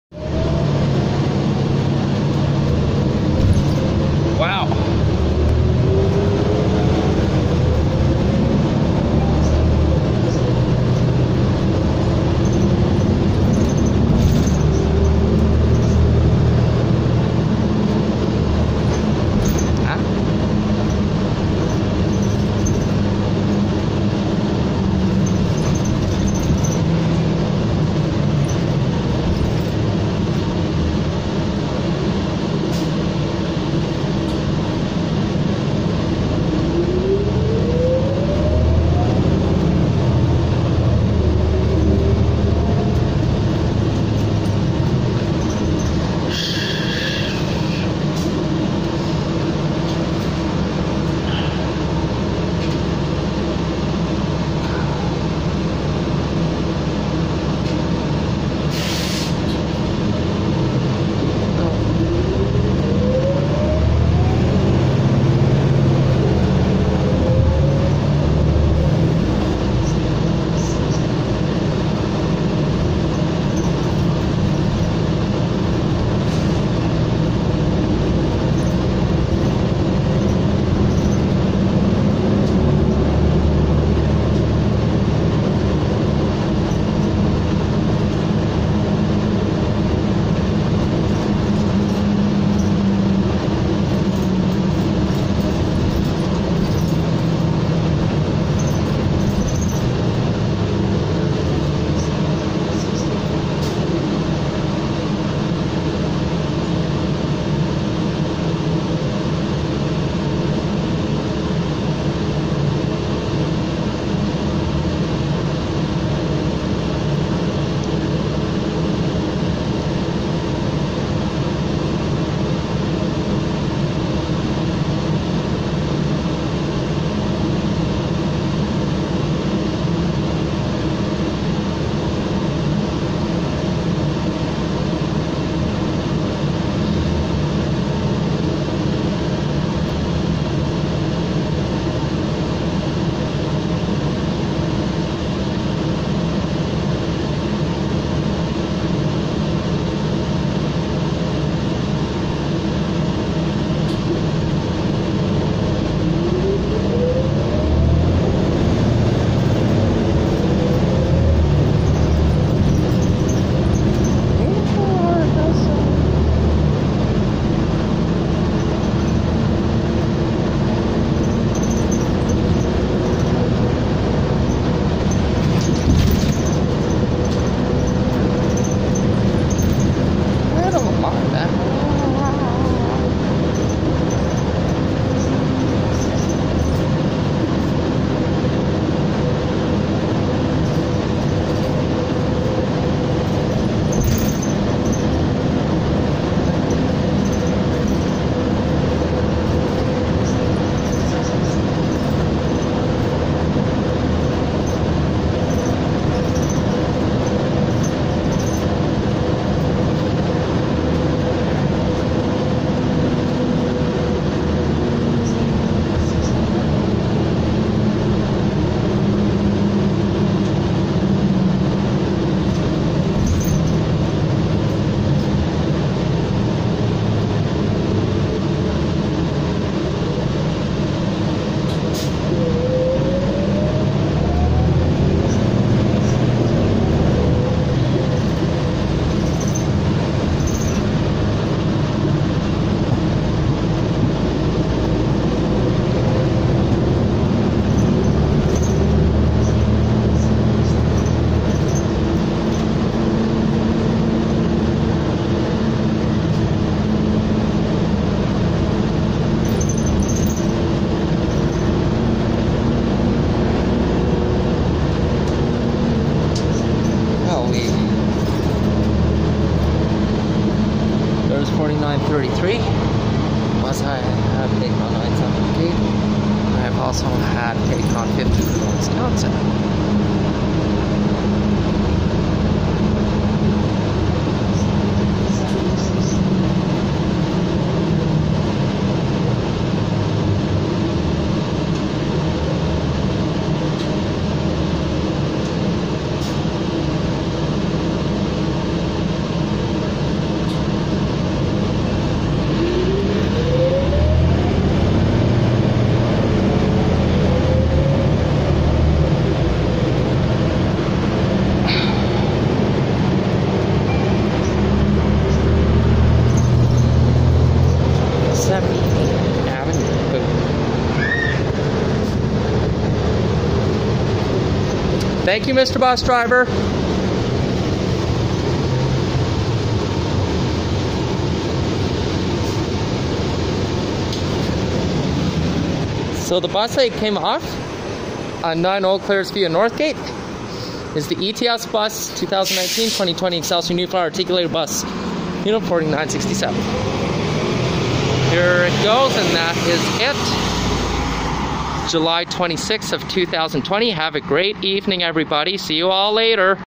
ETS Bus 2019/20 New Flyer XD60 #4967 On 9 Eaux Claires Via Northgate
From 111th Street & 57th Avenue To 109th Street & 78th Avenue in Edmonton Alberta.